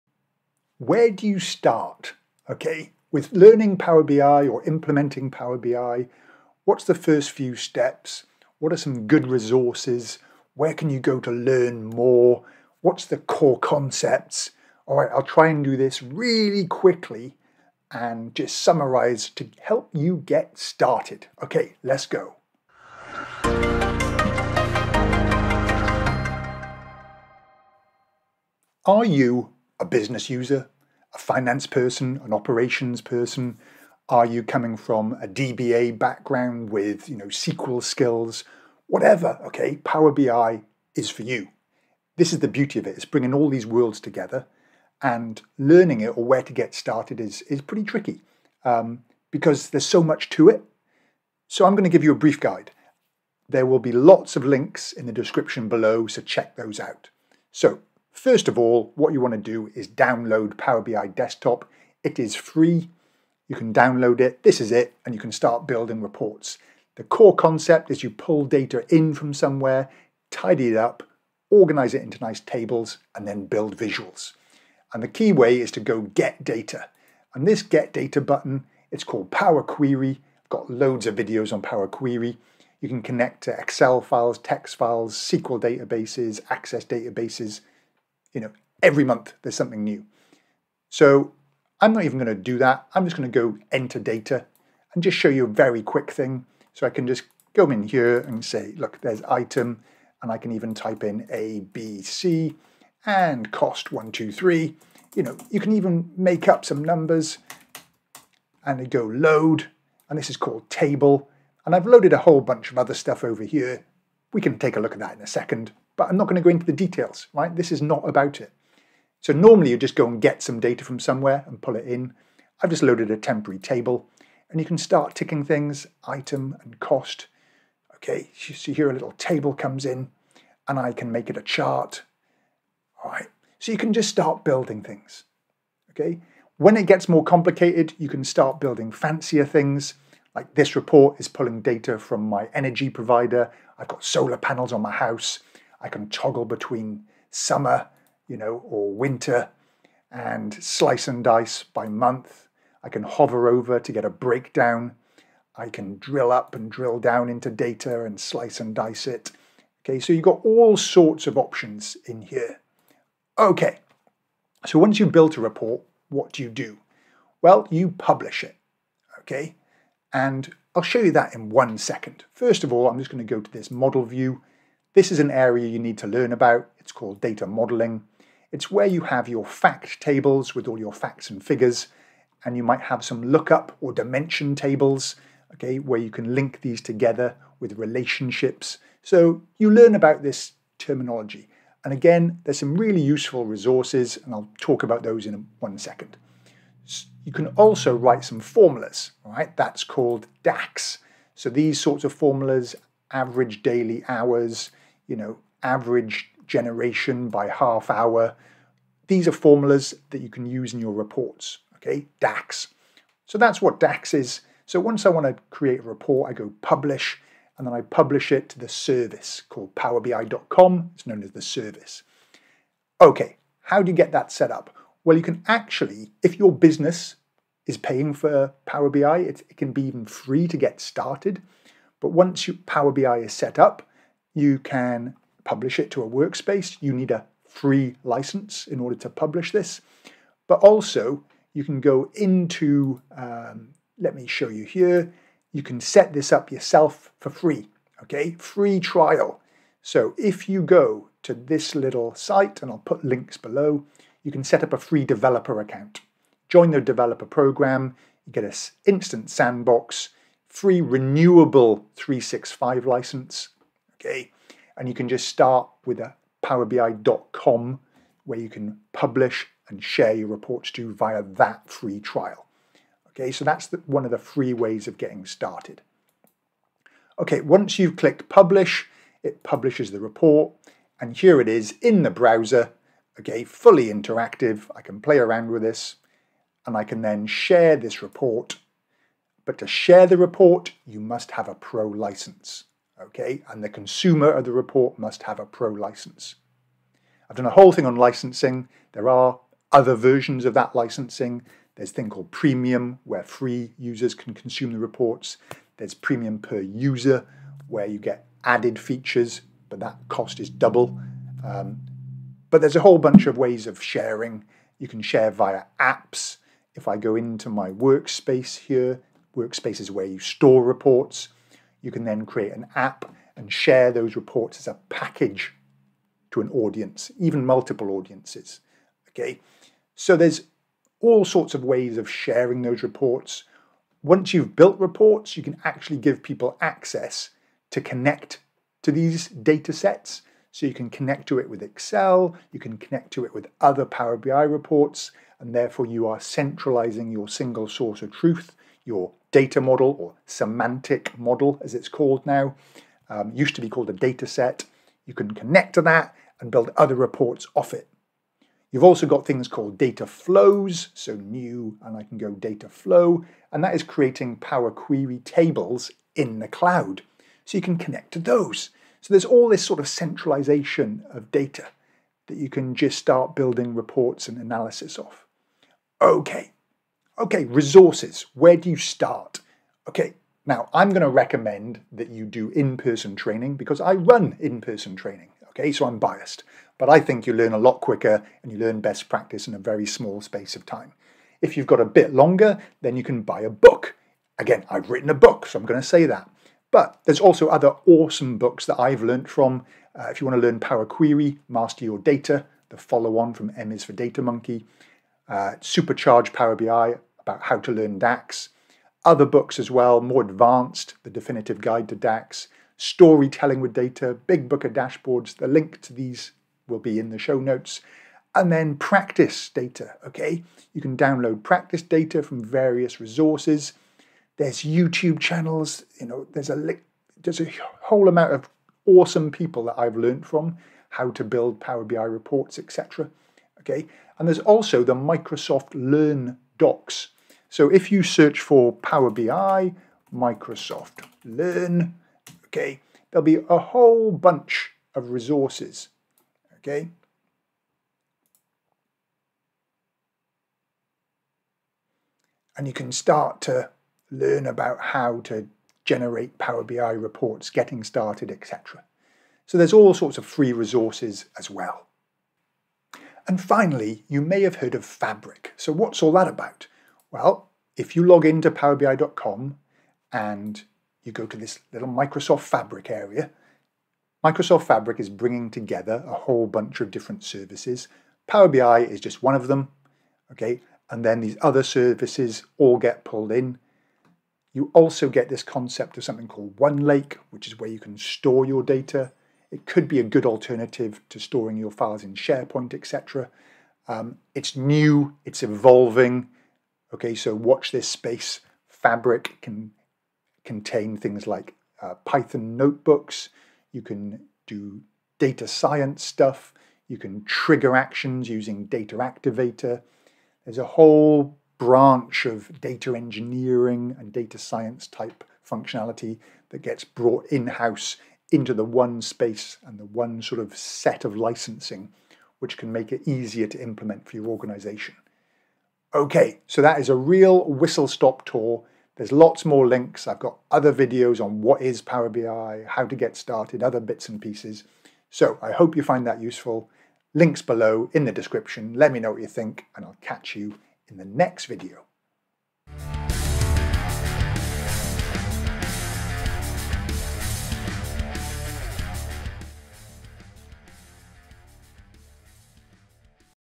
How to get started with Power BI basics
How to get started with Power BI,  what learning resources and key elements do you need to know as a beginner.

1. Download Power BI Desktop

2. The "Service"

3. Free Version for Personal Use (as of March 2024  has sadly been changed to require organisation  account)

4. Resources

5. Community Help

6. Fabric

The team and I at Access Analytic develop Power BI and Excel solutions for clients in Australia and deliver training around the World.